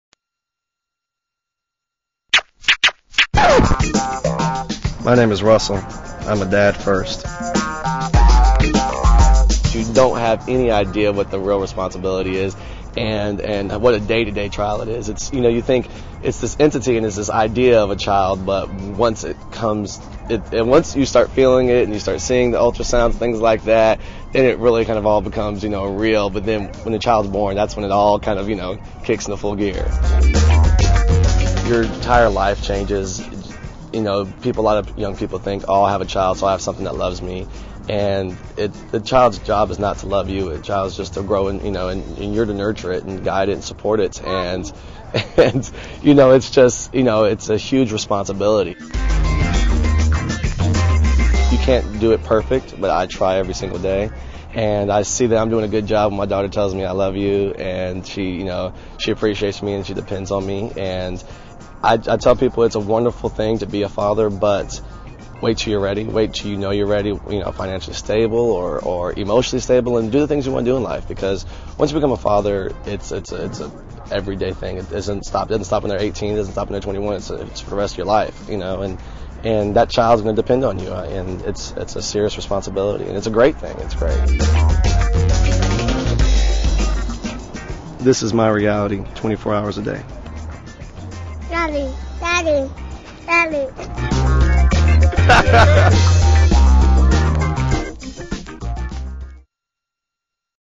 Benefits of Legal Fatherhood
When you establish paternity for a child born to unmarried parents, you are giving that child the benefits of having a legal father, which he or she would not otherwise have.